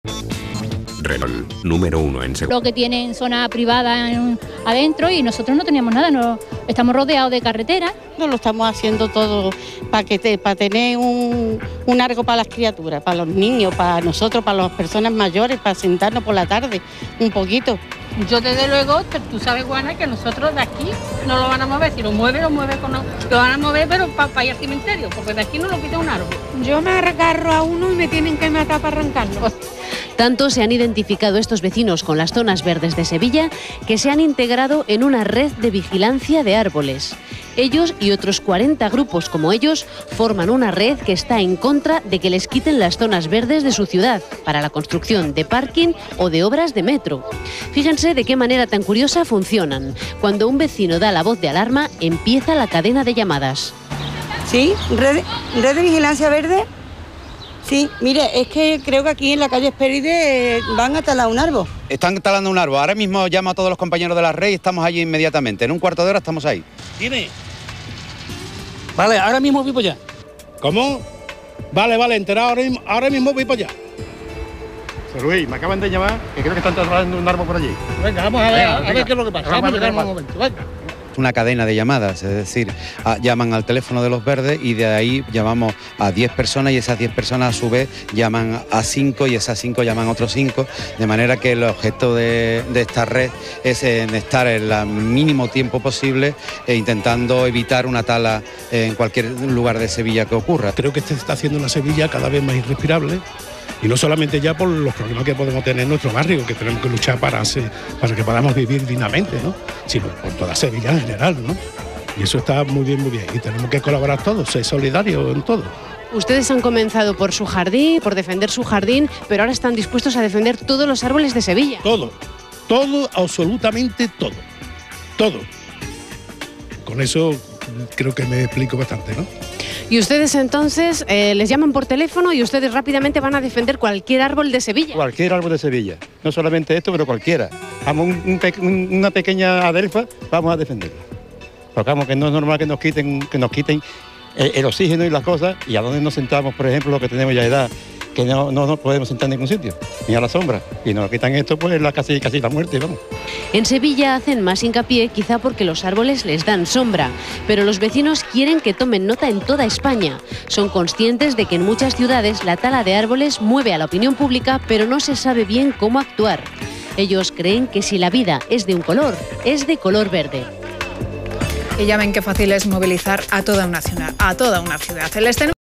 KRI - Arboricidio en Hesperides
En Hesperides contra la tala de arboles. Los vecinos bloque giralda El parque creado por ellos.